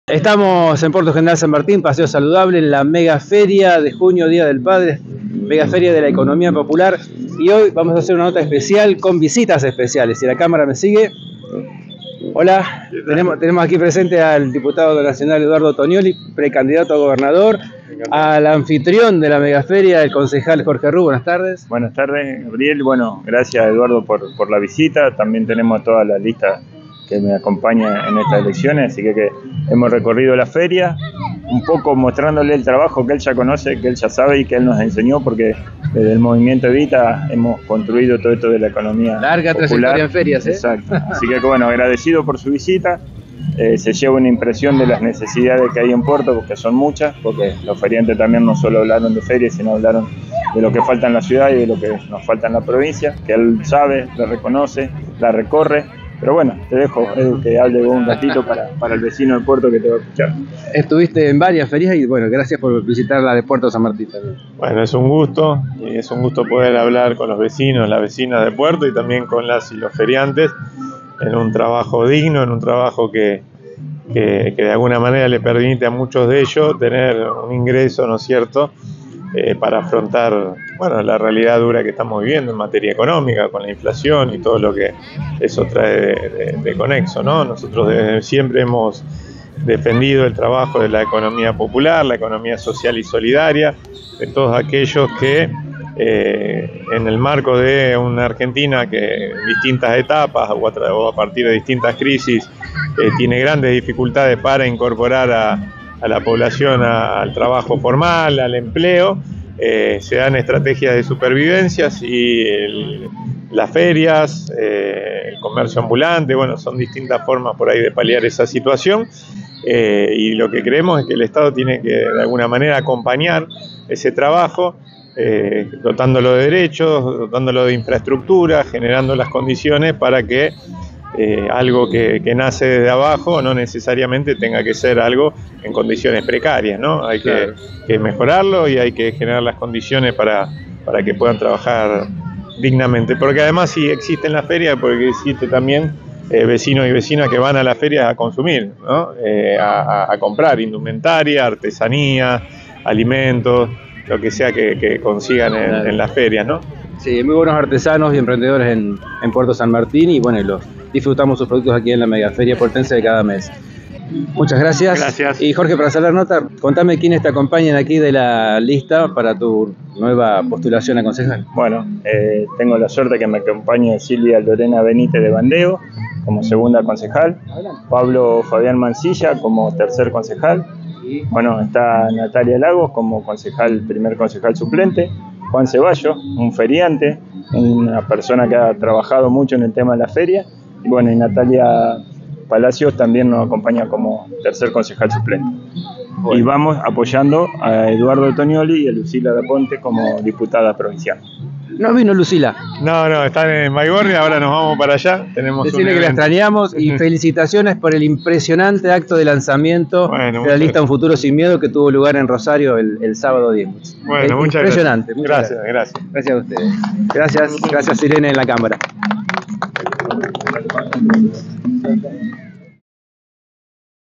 ENTREVISTA CON EDUARDO TONIOLLI Y JORGE RÚ EN LA MEGAFERIA PUERTENSE de la Economía Popular
El Pre-Candidato a Gobernador de Santa Fe EDUARDO TONIOLLI visitó la MegaFeria que cada mes realiza el Frente Puertense Humanista y Social cuyo principal referente es el Concejal JORGE RÚ

Transmitido por el Canal de Videos de NUEVAREGION.COM
Humanismo, No Violencia, Medio Ambiente, Espiritualidad, Actualidad, Opinión y mucho más